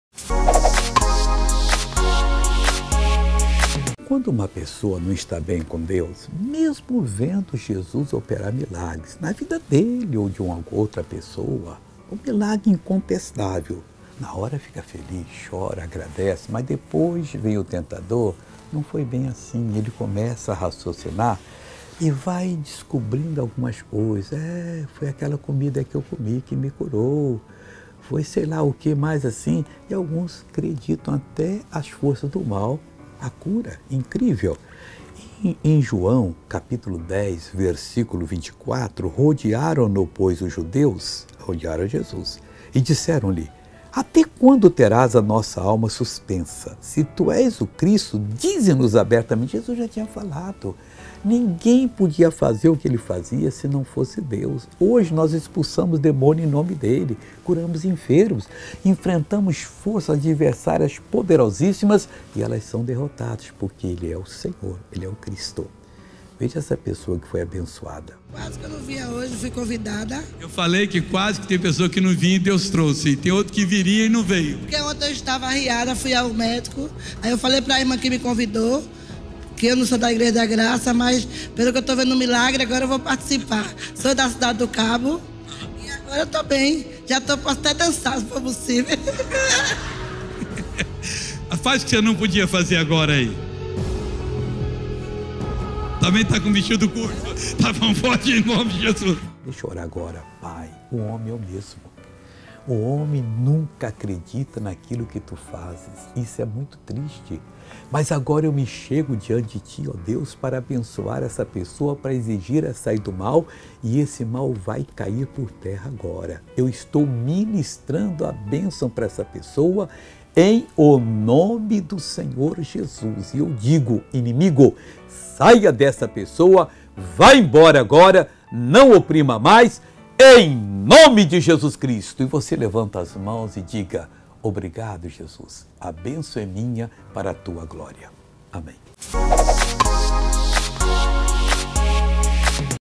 چرا بعضی از انسانھا ایمان نمی آورند؟
خیلی ھا حتی اگر در نام عیسی اشاره ھایی دیده باشند ھم بھ او ایمان نمی آورند. در مقابل بی اعتقادان آگاه
باشید.یوحنا 10: 24